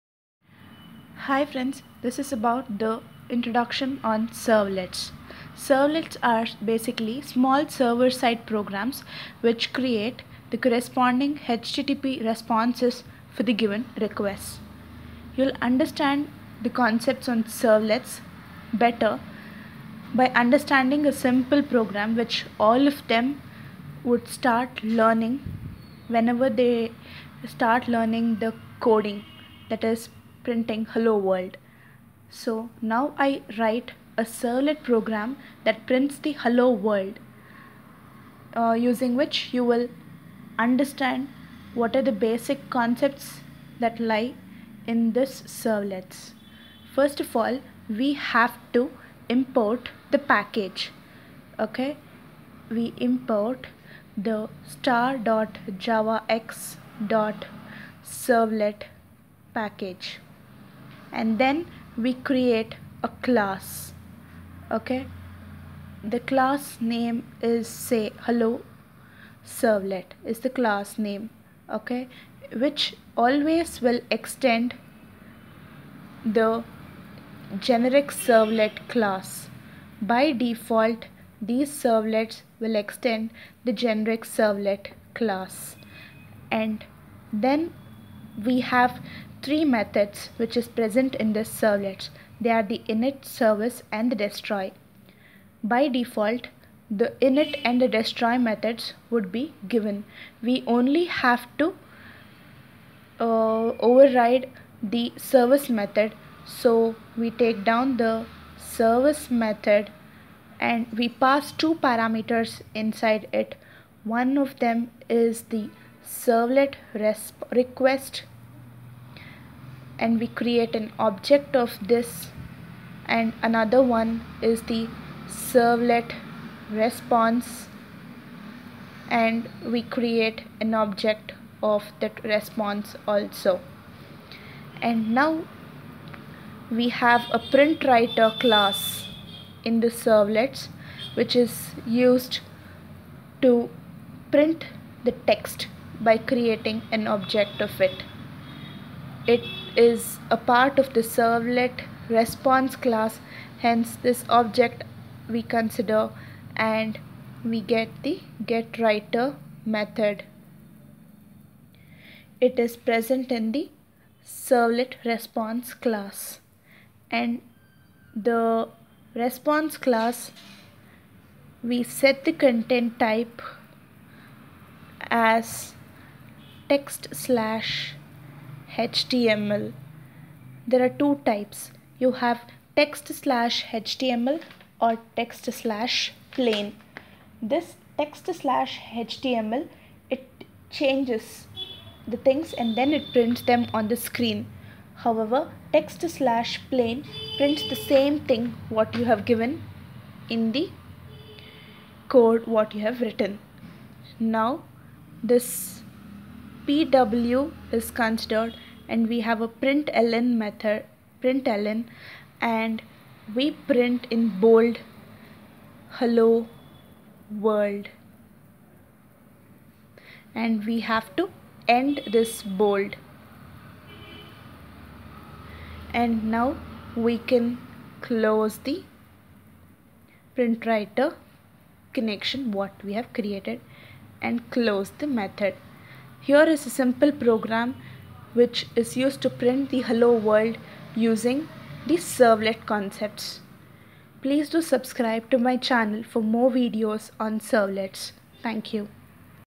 Servlet concepts with a basic "Hello world" program
Servlets is a small server side program that creates the http responses for the given requests.
Job of servlet:
* reads the request
* runs the program to look for      response
* prepares the response